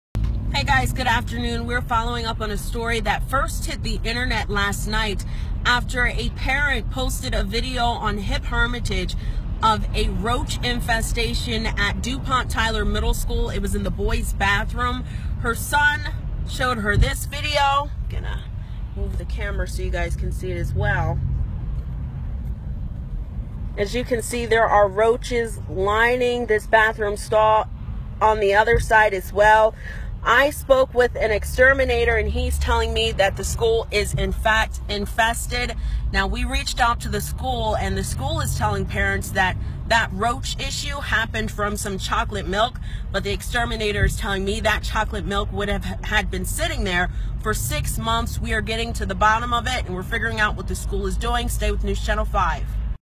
Roaches Caught On Cam At Local Middle School
A student at Dupont Tyler Middle Prep recorded a video of roaches allegedly in the boys' bathroom.